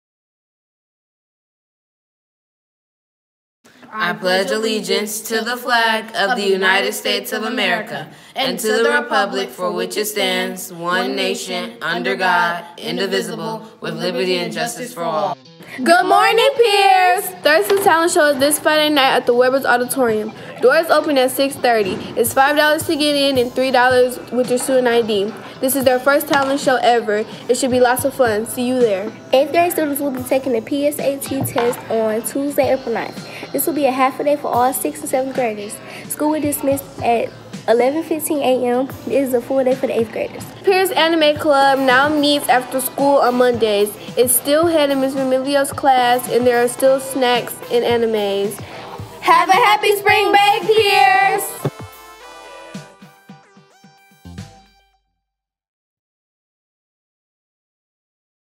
Morning Announcements 03-29-2019
Thurston's talent show is this Friday night at Webber audiorium. Doors open at 6:30. It's $5 to get in or $3 with your student ID. It's the first talent show ever and it should be a lot of fun. See you there!

8th grade students will be taking the PSAT test on Tuesday, April 9.  This will be a 1/2 day for all 6th and 7th graders.  School will dismiss for 6th and 7th graders at 11:15 a.m.  It is a FULL DAY for 8th graders.

The Pierce Anime Club now meets after school on Mondays. It's still held in Ms. Vermiglio's class and there are STILL snacks and Animes.

Have a happy spring break, Pierce!